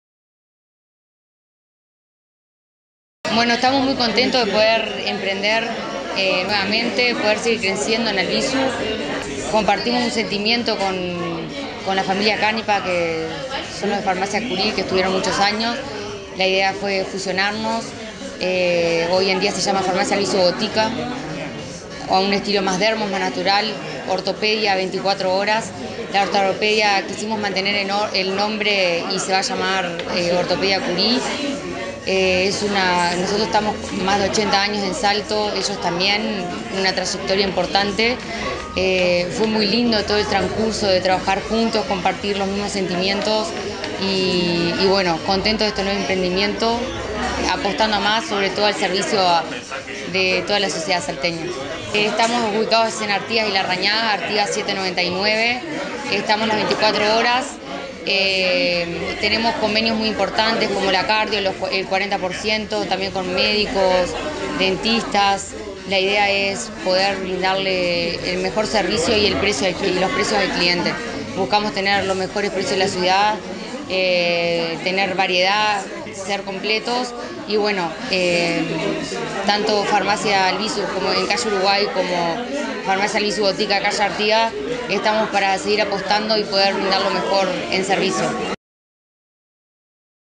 Artigas y Larrañaga- Abrió sus puertas “Farmacia Albisu Botica”
Con un local comercial totalmente remodelado, la esquina de Artigas y Larrañaga ya cuenta con un nuevo espacio de servicios de farmacia y cosmetología. Se trata de “Farmacia Albisu Botica”, una fusión de empresarios del sector que decidieron llevar adelante este emprendimiento donde también ofrecerán otro tipo de servicios a los que ya brindaban.